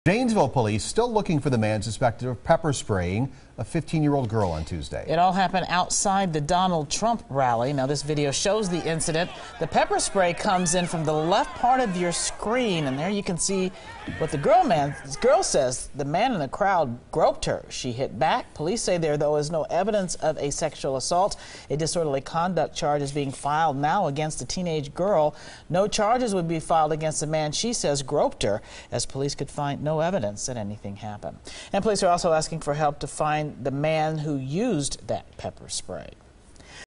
Police: Teen who got pepper sprayed at Donald Trump rally in Janesville wasn't sexually assaulted
15-year-old faces disorderly conduct charges.